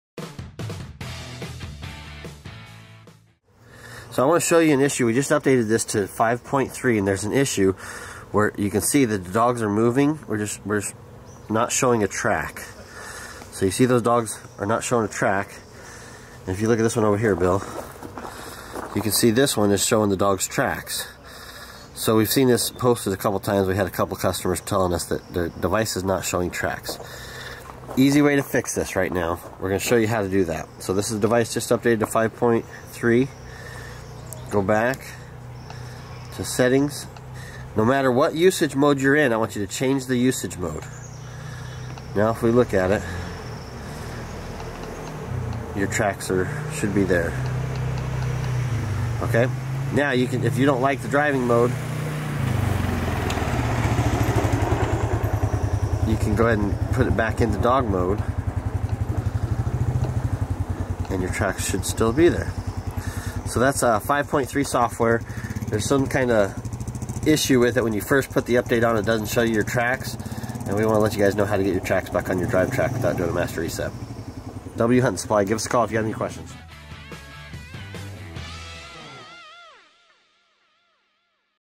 Dog Tracks Not Showing on the DriveTrack After Updating Software
Fix the missing tracks issue in software version 5.3